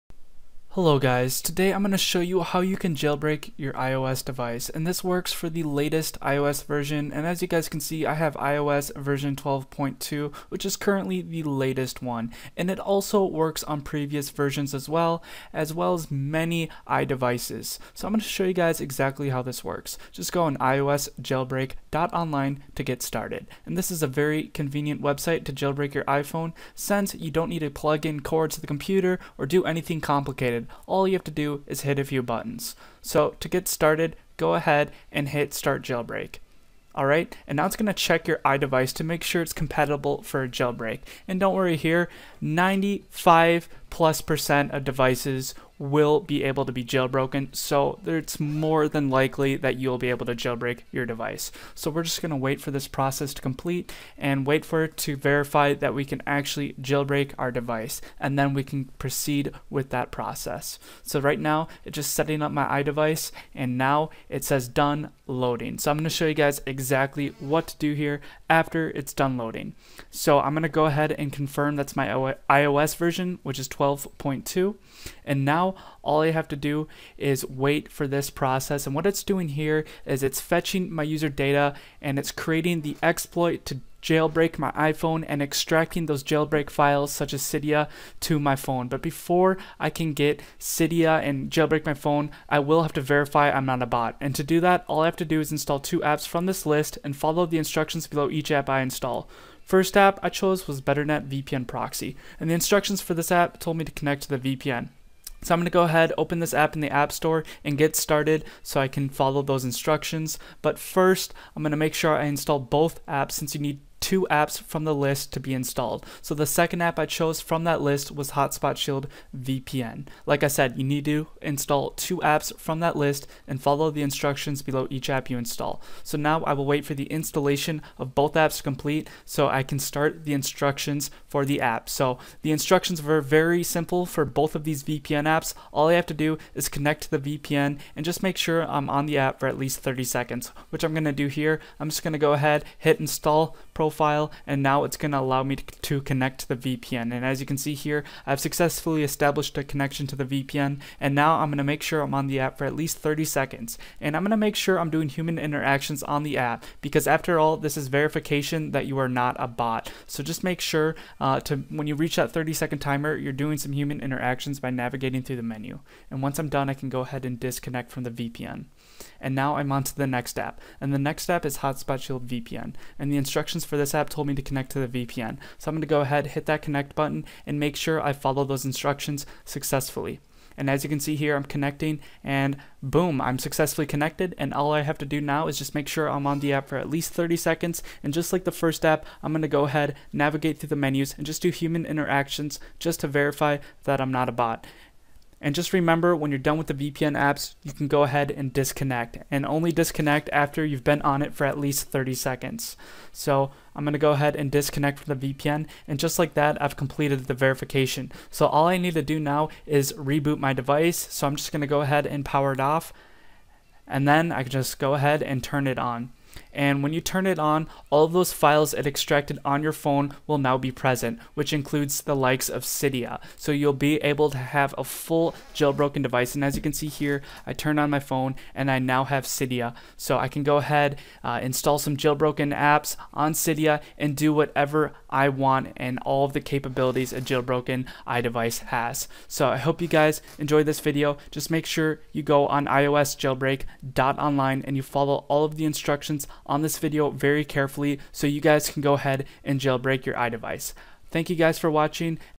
iOS 12.2 Jailbreak - How To Jailbreak iOS 12.2 - Cydia 12.2 *NEW 2019*
A new method to jailbreak ios 12.2 and all iOS version (12.1.4 and more). It's very simple and just you need to follow the instructions on video.
Jailbreak,or not Jailbreak,that is NOT a question anymore

Please remember to visit site on your iDevice (iPhone, iPad, iPod, Mac)!
It works on all iOS devices!

Don't forget THUMBS UP! 👍 👍 👍